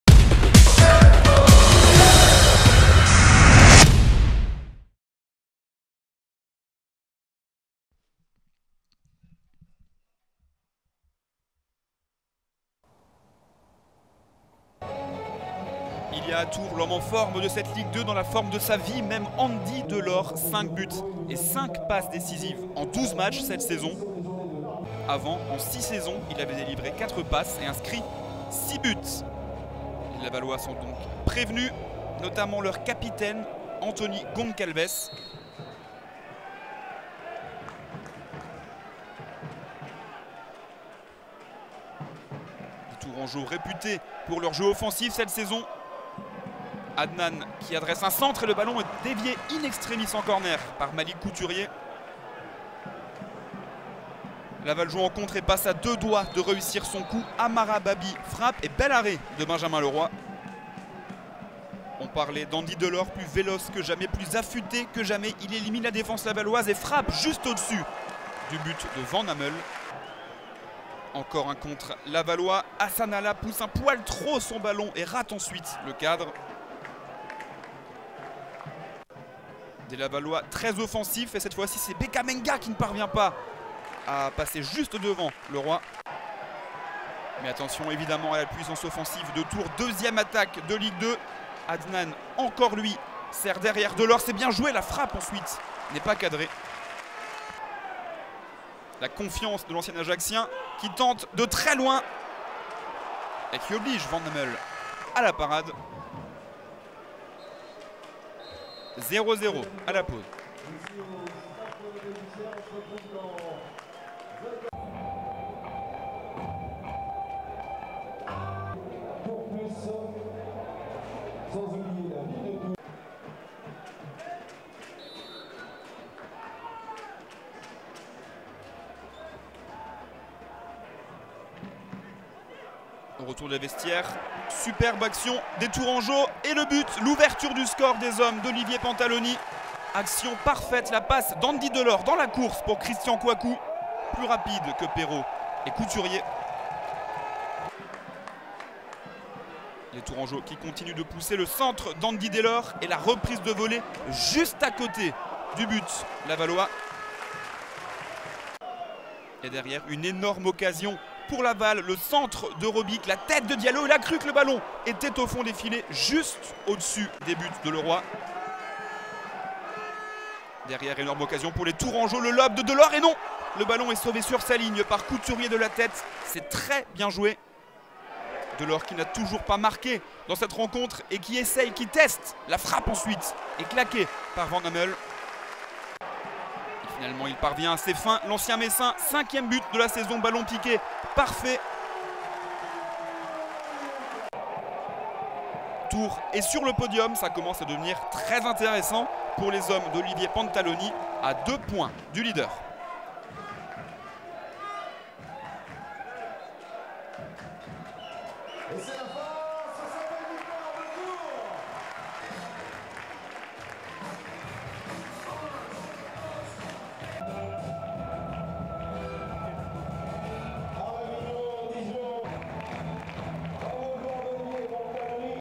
Tours FC - Stade Lavallois (2-0) - Le résumé (TOURS - LAVAL) - 2013/2014
Revivez les meilleurs moments de Tours FC - Stade Lavallois (2-0) en vidéo.

Stade de la Vallée du Cher - vendredi 25 octobre 2013

Buteurs : Christian KOUAKOU (50' - Tours FC) / Andy DELORT (81' - Tours FC)

Composition Tours FC : 2 - Fousseni DIAWARA / 7 - Pascal BERENGUER / 8 - Kévin DIAZ / 9 - Andy DELORT / 10 - Christian KOUAKOU / 15 - Thomas FONTAINE / 21 - Youssef ADNANE / 22 - Léo SCHWECHLEN / 25 - Samuel BOUHOURS / 28 - Julien CETOUT / 30 - Benjamin LEROY (c)

Composition Stade Lavallois : 2 - Kévin PERROT / 6 - Anthony GONCALVES (c) / 8 - Amara BABY / 11 - Mamadou DIALLO / 12 - Jordan ADEOTI / 13 - Miodrag STOSIC / 14 - Guillaume RIPPERT / 18 - Hassane ALLA / 21 - Malik COUTURIER / 23 - Antony ROBIC / 30 - Mike VANHAMEL